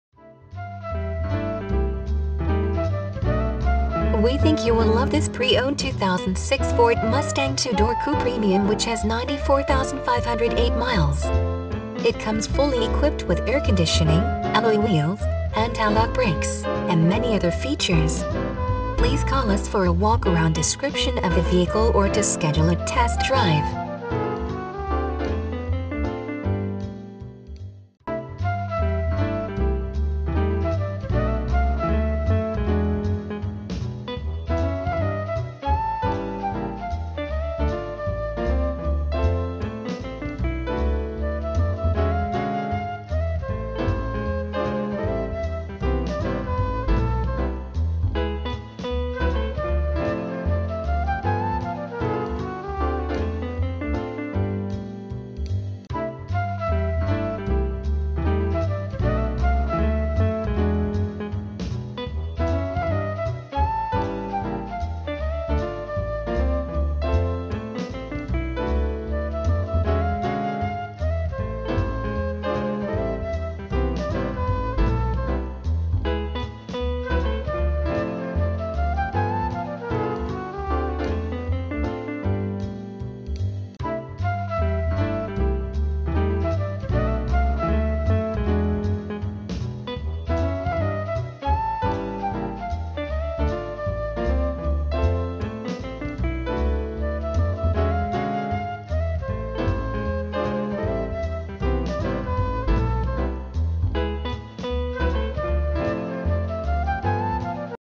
2006 FORD MUSTANG Premium Manual 4.0L Yellow
2006 FORD Mustang 2 Door Coupe Premium

http://

2006 FORD MUSTANG Premium Manual 4.0 Liter Yellow, ABS, AC, AM/FM, CD, alloy wheels, child safety locks, dual front air bags,lumbar support, power locks, steering, windows, seat, mirrors, TCS, rear spoiler, premium sound, Pony Pkg. Come in for a test drive today or visit us online at longmontkia.com

Longmont Kia
Longmont, CO